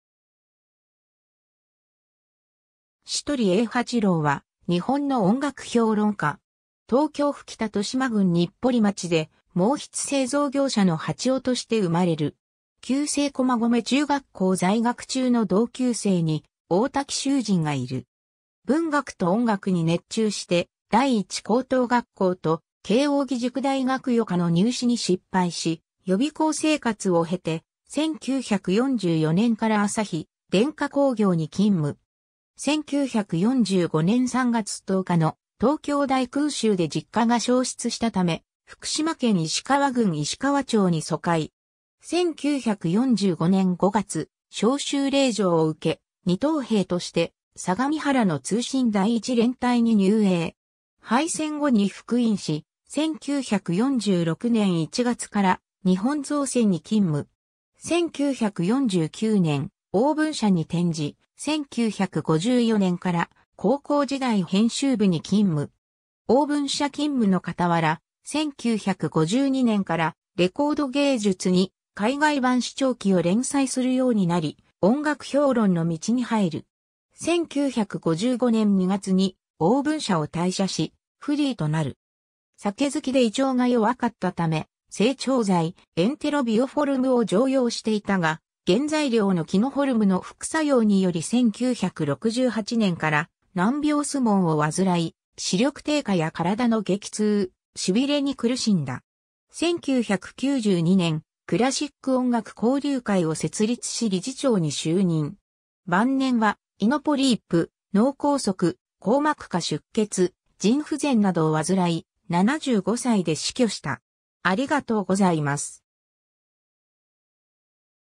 志鳥栄八郎
志鳥 栄八郎（しどり えいはちろう、1926年1月24日 - 2001年9月5日）は日本の音楽評論家。
 東京府北豊島郡日暮里町（現・東京都荒川区日暮里）で毛筆製造業者の八男として生まれる。
旧制駒込中学校（現・駒込中学校・高等学校）在学中の同級生に大滝秀治がいる。
文学と音楽に熱中して第一高等学校 (旧制)と慶應義塾大学予科の入試に失敗し、予備校生活を経て、1944年から旭電化工業に勤務。
1945年3月10日の東京大空襲で実家が焼失したため、福島県石川郡石川町に疎開。
1945年5月、召集令状を受け、二等兵として相模原の通信第一連隊に入営。
敗戦後に復員し、1946年1月から日本造船に勤務。
1949年、旺文社に転じ、1954年から『高校時代』編集部に勤務。
旺文社勤務のかたわら、1952年から『レコード芸術』（音楽之友社）に海外盤試聴記を連載するようになり、音楽評論の道に入る。
1955年2月に旺文社を退社し、フリーとなる。
 酒好きで胃腸が弱かったため、整腸剤エンテロ・ヴィオフォルムを常用していたが、原材料のキノホルムの副作用により1968年から難病スモンを患い、視力低下や身体の激痛、痺れに苦しんだ。
1992年、クラシック音楽興隆会を設立し理事長に就任。
晩年は胃のポリープ、脳梗塞、硬膜下出血、腎不全などを患い、75歳で死去した。